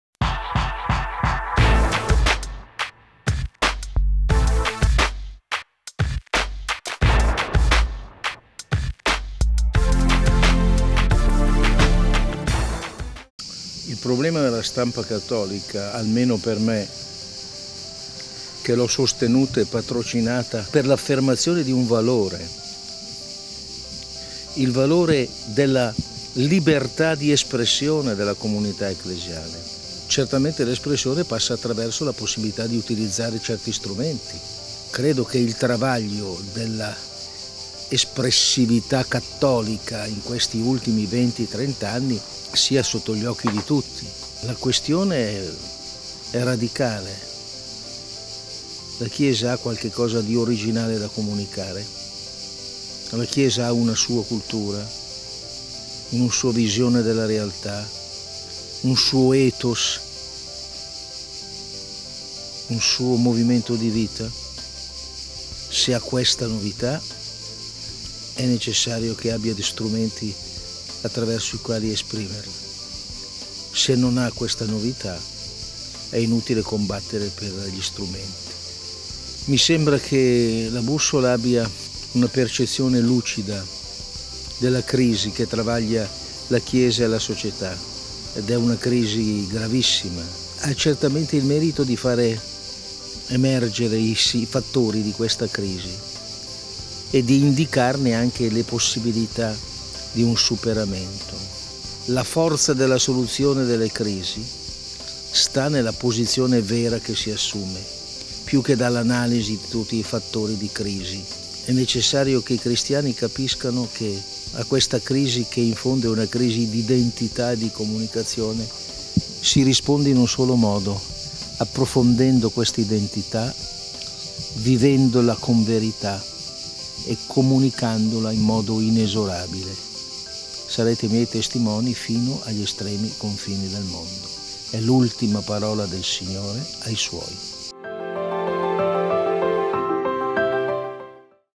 Mons Negri e la stampa cattolica
«E' necessario che i cristiani capiscano che l'evidente e profonda crisi che la società e la Chiesa stanno attraversando, è una crisi di identità.  C'è un solo mondo per rispondere: approfondendo questa identità, vivendola con Verità e comunicandola in modo inesorabile. "Sarete miei testimoni fino agli estremi confini del mondo", è l'ultima parola del Signore ai suoi. La Bussola è un lucido ed intelligente tentativo di rendere questa testimonianza».